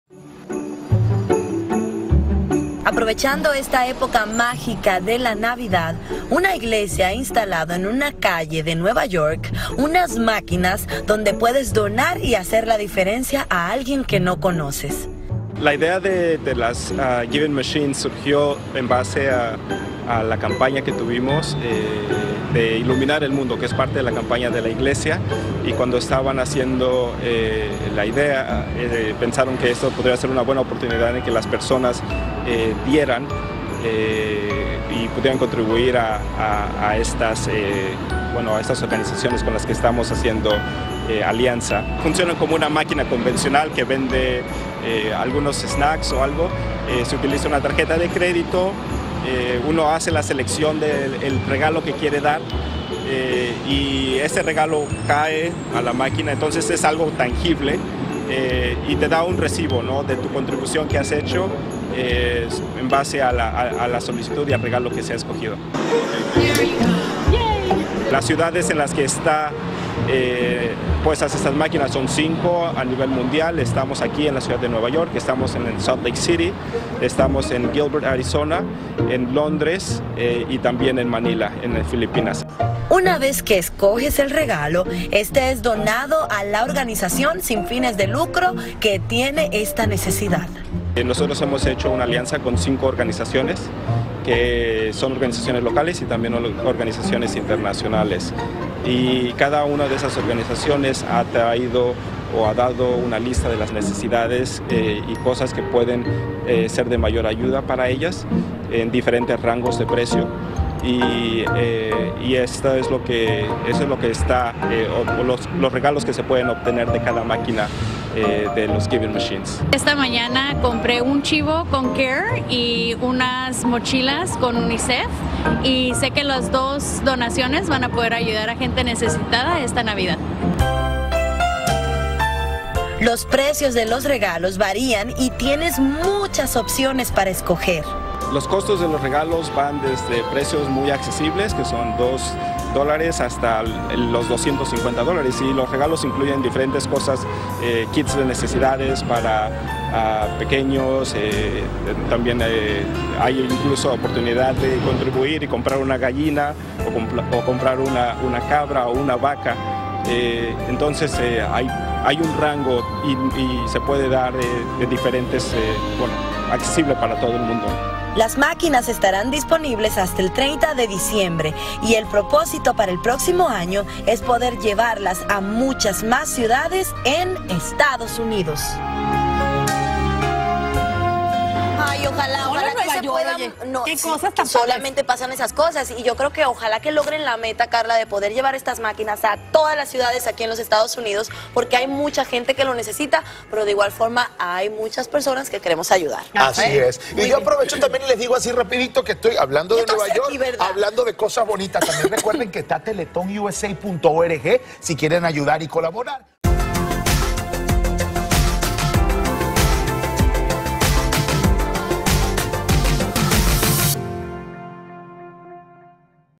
The Giving Machine: la nueva modalidad para darle un regalo a un desconocido
En la Gran Manzana existe una nueva manera de alegrarle el día a alguien más: una máquina expendedora de regalos de todo tipo. ¿Un desconocido necesita una manta para cubrirse del frío? Ahí puedes comprarla. ¿Comida? También está disponible. Esta Navidad ya no hay excusa para brindar un obsequio.